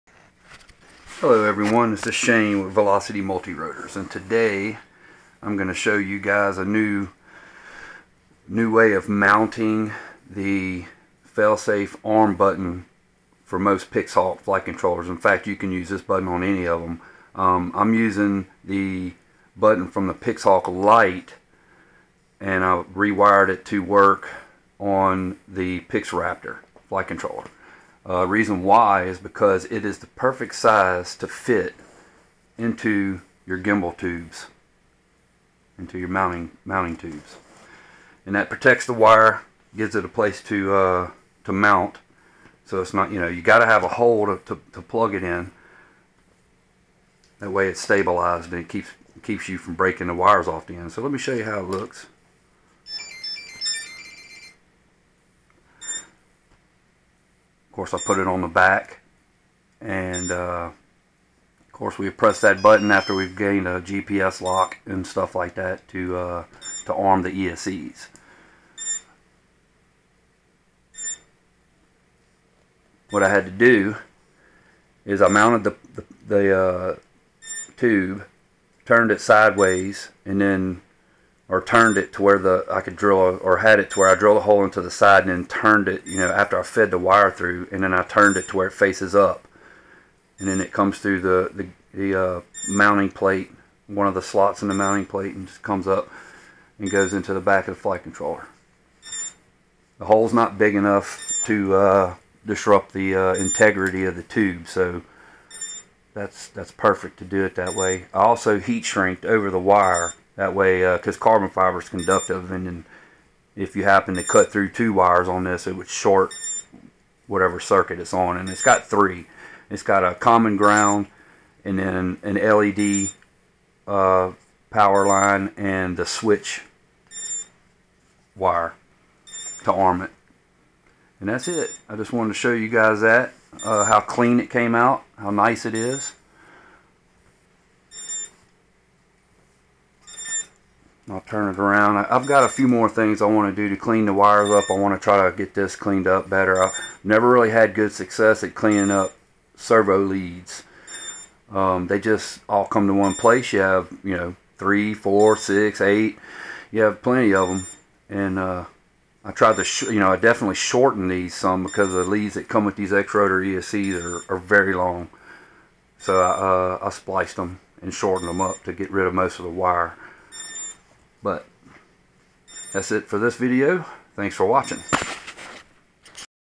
Mounting The Failsafe Switch For the Pixhawk Flight Controllers.
Here is a great way I found to mount the fail safe arming switch for ALL Pixhawk flight controllers. As we all know by now the Pixhawk is very different from APM in how it arms the ESC's. This takes a momentary switch that must be pressed to arm the ESC's. Finding a place to mount these can be a pain and they all have a set of wires that have to connect. We have to protect these wires and the switch needs to be easily accessible. I thought about this for a while and came up with this. So for people that have tubes for mounting gimbals which 99% of them, I've seen have an I.D. that is perfect for the round hole through fail-safe switches.